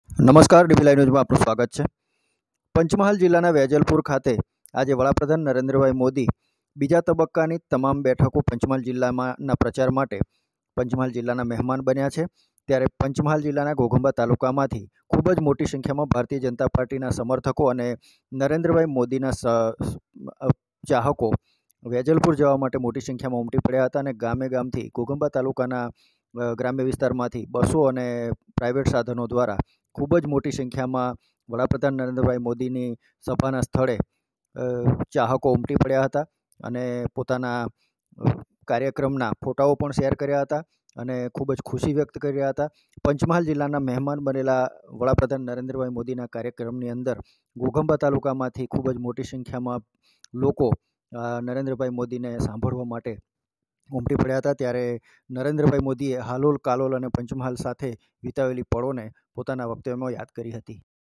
વેજલપુર ખાતે આવેલા વડાપ્રધાન મોદીજી ના કાર્યક્રમમાં ઘોઘંબા તાલુકાના સમર્થકો ની ભારે ભીડ
વેજલપુર ખાતે આવેલા વડાપ્રધાન મોદીજી ના કાર્યક્રમમાં જવા માટે ઘોઘંબા માંથી મોટી સંખ્યા માં લોકો રવાનાવડાપ્રધાન જાહેરસભા વેજલપુર માં મોદીdbliveghoghamba